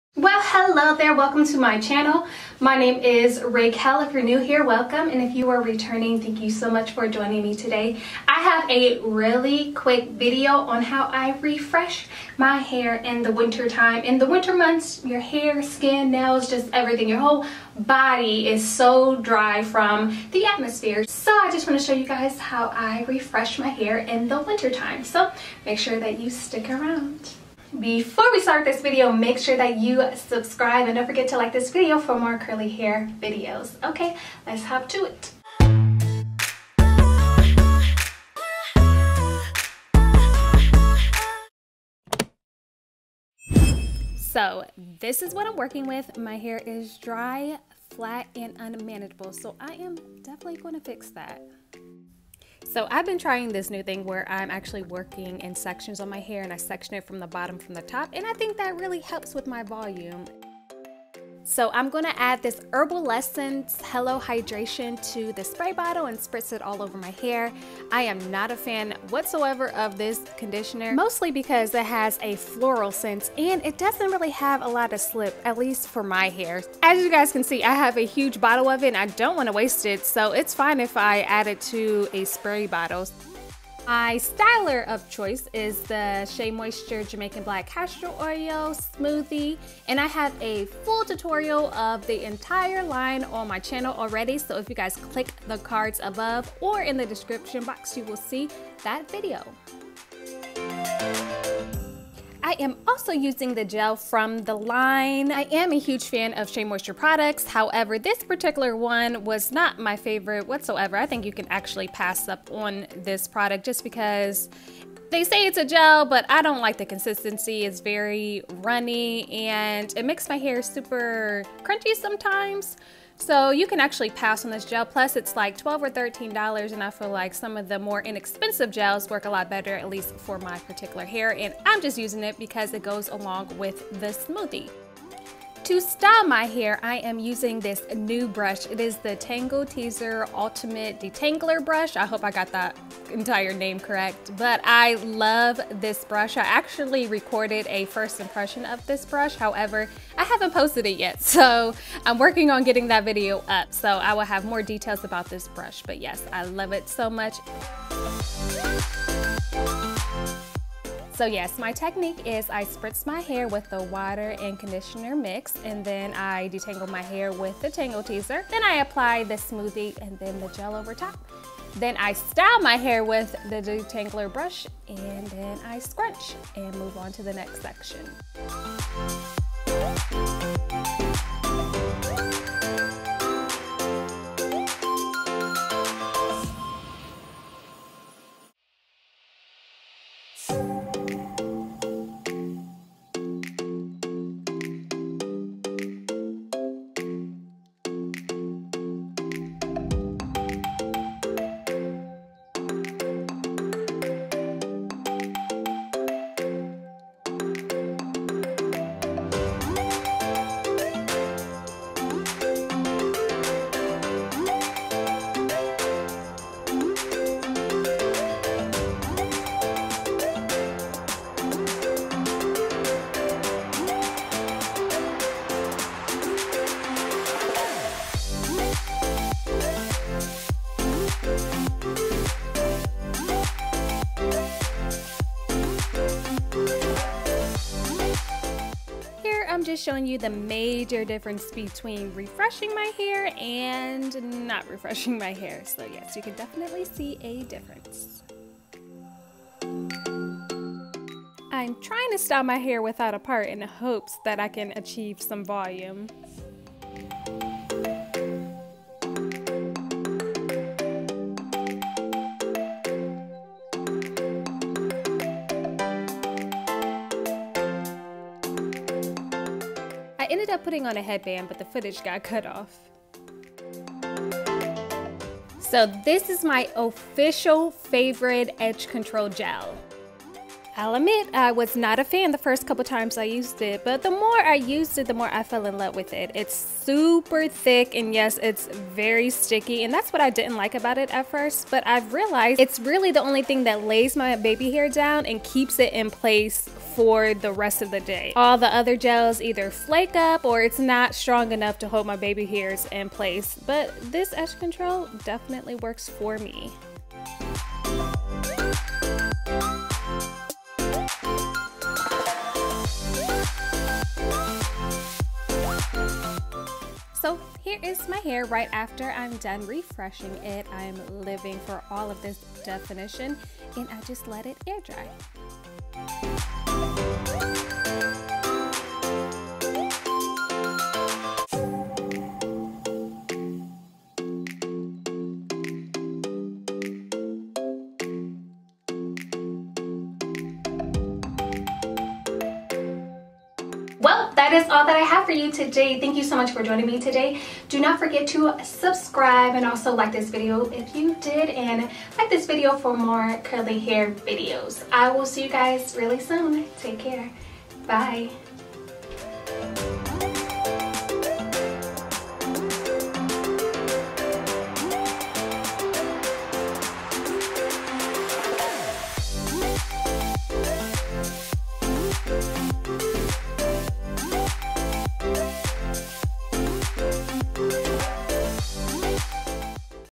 HOW TO: REFRESH OLD WASH AND GO| Curly Hair Routine | Winter 2019
Here is how I refresh my curly hair during the Winter. Thumbs up for more curly hair videos!

Products Used:
Herbal Essence Hello Hydration Conditioner
Shea Moisture Jamaican Black Castor Oil Smoothie
Shea Moisture Jamaican Black Castor Oil Gel
Shea Moisture Jamaican Black Castor Oil Edge Control

Tools Used:
Tangle Teezer Ultimate Detangler Brush